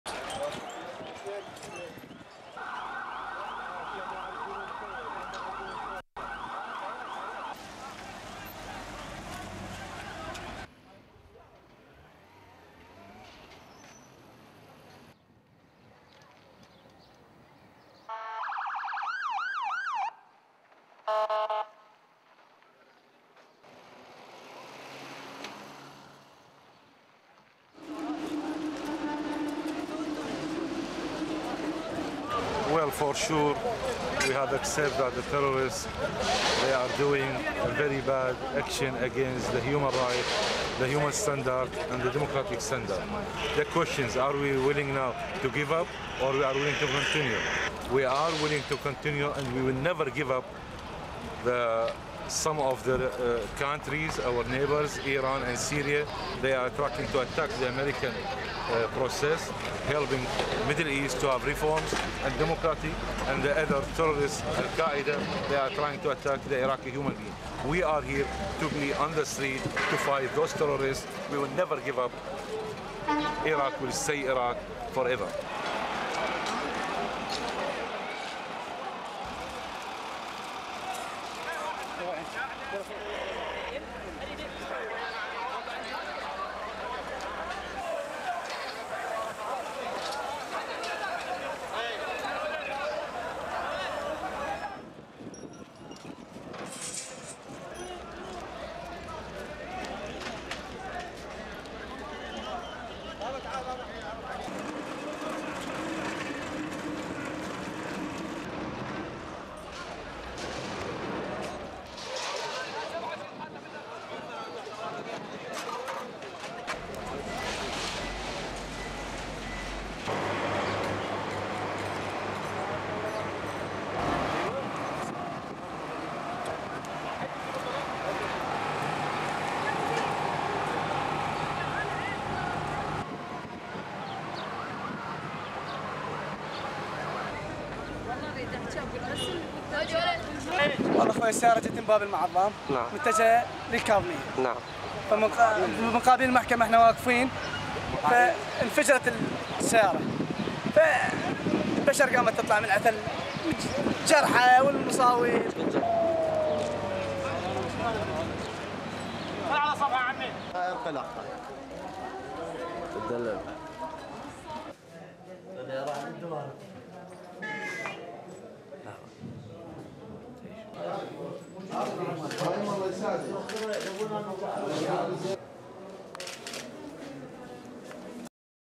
WRAP Minibus bomb kills 11, suicide bomber kills at least 8 on another bus
Karradah neighbourhood
1. Various of scene of the blast
2. Ambulance arriving at the scene
3. Wide of street
4. Sign reading: (Arabic) Ibn al-Nafis Teaching Hospital
5. Ambulance driving to hospital (UPSOUND) siren
6. Two young men helping injured man across street
7. Wide pan of scene of the blast
8. SOUNDBITE: (English) Mithal al-Ilusi, Iraqi lawmaker:
"For sure, we have accepted that the terrorists are doing very bad actions against human rights, human standards and democratic standards. The question is, are we now willing to give up or are we willing to continue? We are willing to continue and we will never give up. Some of the countries, our neighbours Iran and Syria, they are plotting to attack the American process (which is) helping the Middle East to have reforms and democracy. And the other terrorists, Al-Qaida, they are trying to attack the Iraqi human beings. We are here, on the street, to fight those terrorists. We will never give up. Iraq will stay Iraq forever."
9 . Various of blast scene
U'tayfiyah neighbourhood
10. Damaged minibus
11. Damage inside the bus
12. Burnt out minibus
13. Damage inside the bus
14. Various of wreckage of minibus blown up by suicide bomber
15. SOUNDBITE: (Arabic) Mohammed Jassim, Policeman
"A minivan coming from Bab al-Mu'dham neighbourhood heading to Kazimiyah exploded here while we were standing in front of a courthouse. The blast left so many casualties."
16. Wide of the blast scene
Yarmouk neighbourhood
17. Men pushing an injured person on a stretcher to hospital
18. Paramedics treating injured people
19. Various of injured lying on bed with their relatives looking after them
20. An injured person being treated by medical staff
21. An injured man lying on a bed with a bandaged arm
STORYLINE:
Two separate attacks on minibuses in the Iraqi capital Baghdad killed 19 people and injured 26 others on Sunday.
A bus rigged with explosives detonated in the busy commercial area of Karradah, killing 11 and wounding 15, according to police and hospital officials.
Witnesses said the explosion also damaged nearby cars and shops.
Mithal al-Ilusi, an Iraqi lawmaker, visited the scene of the Karradah bombing and tried to reassure local people.
"We are here, on the street, to fight those terrorists. We will never give up. Iraq will stay Iraq forever," he said
The blast came just hours after a suicide bomber blew himself up on a minibus in northwest Baghdad, killing at least eight people and wounding 11 others, officials said.
The blast ripped through the minivan shortly after noon near a courthouse in the mainly Shiite neighbourhood of al-U'tayfiyah.
An official at the Kazimiyah Hospital said many of the victims were severely burned.
The minibus explosions were the latest incidents in a series of deadly bombings in the past two days.
Earlier on Sunday, two car bombs exploded five minutes apart in a busy market district, killing at least 18 people and wounding 50.
In all, six bombs exploded in predominantly Shiite sections of the capital on Sunday in a renewal of sectarian carnage.
The spate of bombings comes despite a US security crackdown aimed at stopping insurgent attacks.